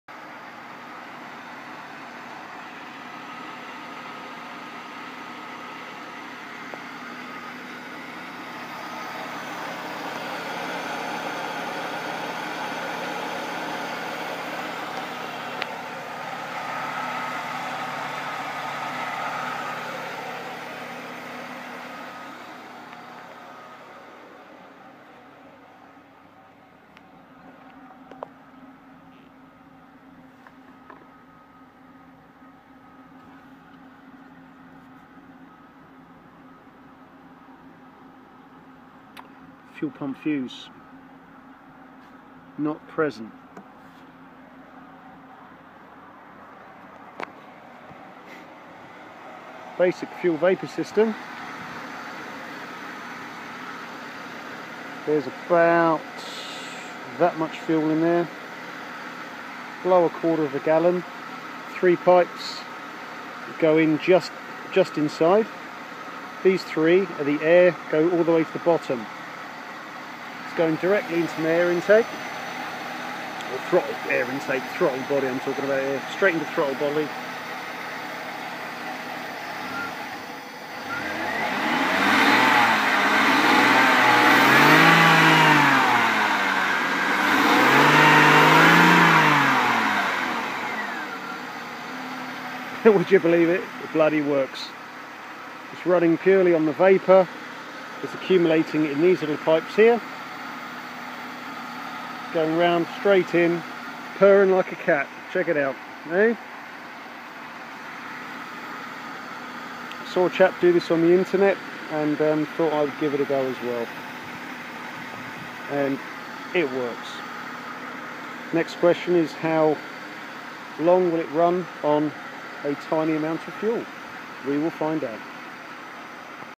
Car Running On Petrol Vapour / Fumes / Jaguar X300 4.0 Sport
A basic hillbilly bodge vapour system copied from other you tube videos.  My Jag had knackered injectors and was a non runner so a great chance to check my coils and spark as well as curiosity. Runs fine at idle and revving relatively normally.  Popped into drive and it did not stall, slightly lumpy engine observed.  Fuel pump disconnected, fuel rail confirmed dry.  5th of Gallon on petrol in 1 gallon can.  Really exciting, ran for over half an hour, switched it off myself.  Still loads of fuel left and coloured slightly white, imagine moisture / air contaminant. Adjustments to follow, 2 gallon can, valve to adjust air/fuel mix.  Flashback protector installed at throttle intake (Steel mesh strainer cannibalised) provides basic protection.  Working ultimately towards designing a very sturdy system that can fit into engine bay safely and be fully regulated.